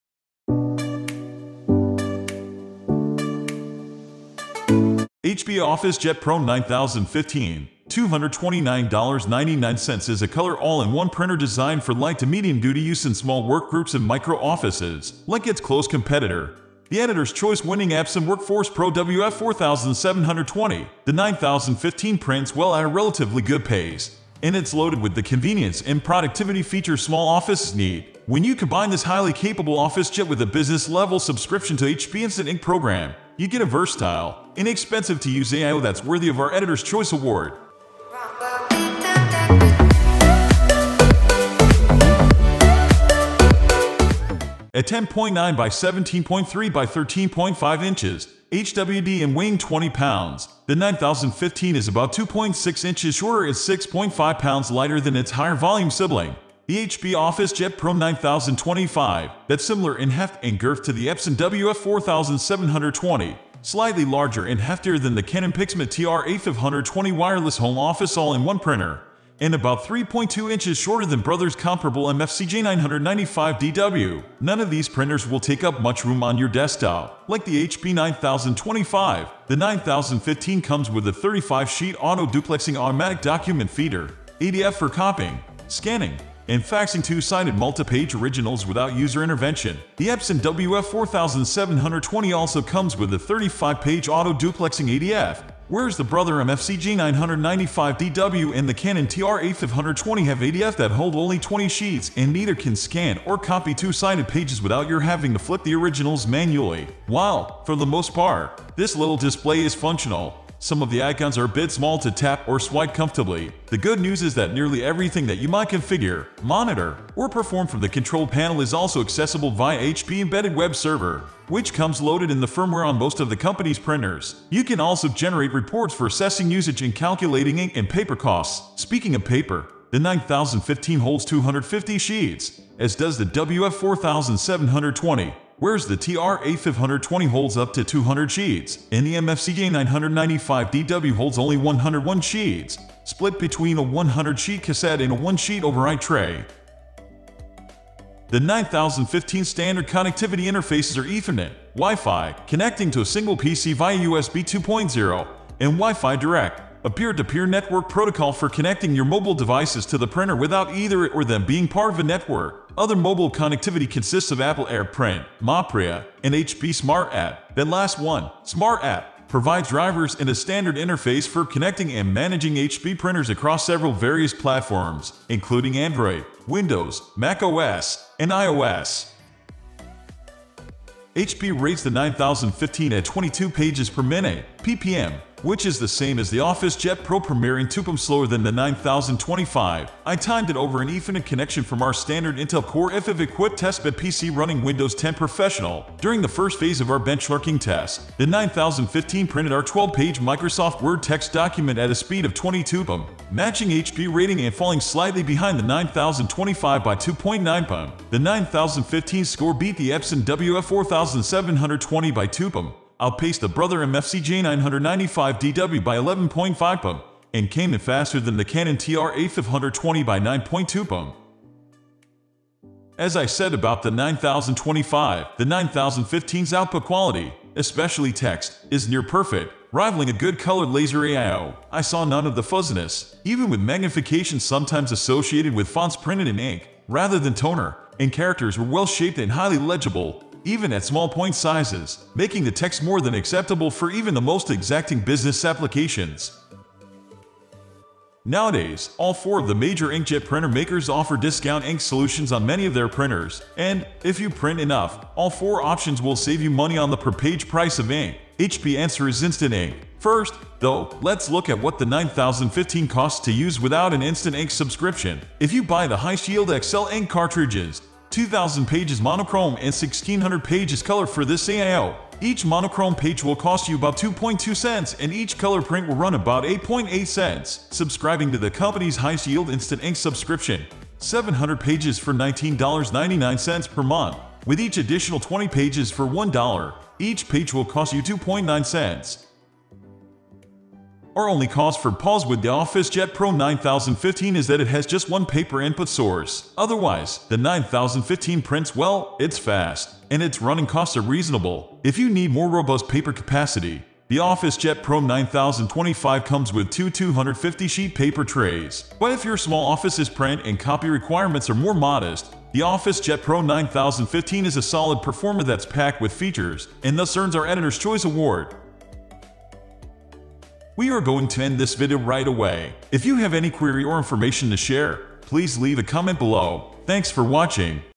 HP OfficeJet Pro 9015 All-in-One Printer Review: Is It Any Good?!
HP's OfficeJet Pro 9015 is a color all-in-one printer designed for light- to medium-duty use in small workgroups and micro offices. Like its close competitor, the Editors' Choice-winning Epson WorkForce Pro WF-4720, the 9015 prints well at a relatively good pace, and it's loaded with the convenience and productivity features small offices need.